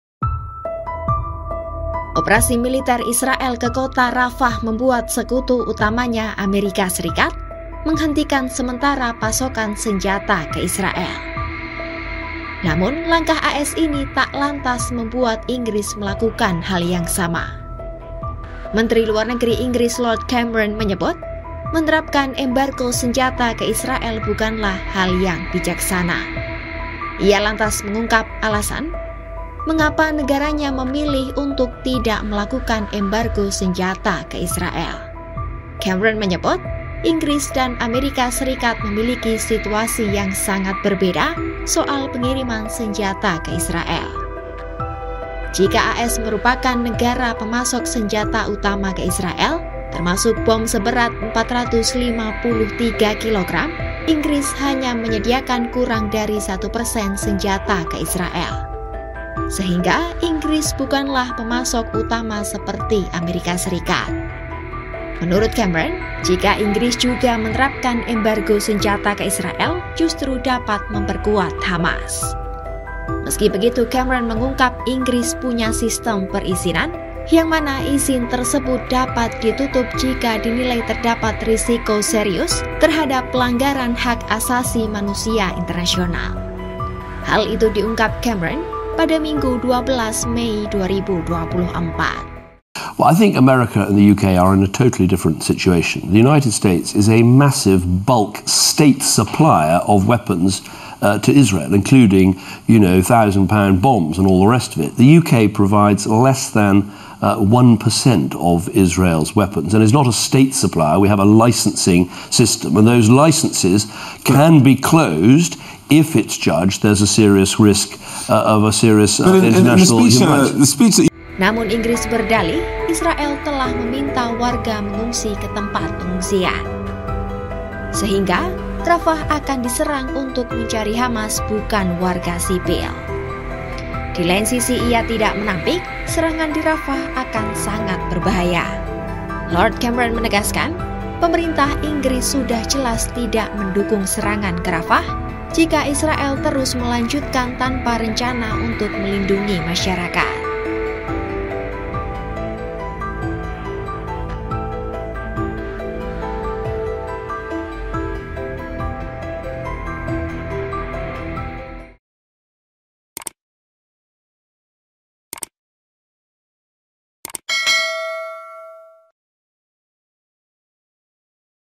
Inggris Sebut Embargo Senjata ke Israel Dapat Perkuat Hamas
Operasi Israel ke Kota Rafah membuat Amerika Serikat menghentikan sementara pasokan senjata ke Israel. Namun Inggris memilih untuk tidak melakukan hal yang sama.

Menteri Luar Negeri Inggris, Lord Cameron mengungkap alasan mengapa negaranya memilih untuk tidak melakukan embargo senjata ke Israel.

"Embargo senjata Israel bukanlah jalan yang bijaksana dan Inggris serta Amerika berada dalam situasi yang sangat berbeda," kata Lord Cameron kepada Sunday Morning With Trevor Phillips Show.

Lantas apa yang membuat Inggris tak mengikuti jejak AS dan menghentikan pasokan senjata ke Israel? Simak selengkapnya dalam video berikut!

Penulis: Albertus Adit
Penulis Naskah: Meiva Jufarani
Narator: Meiva Jufarani
Video Editor: Tri Febrianto Gunawan
Produser: Adisty Safitri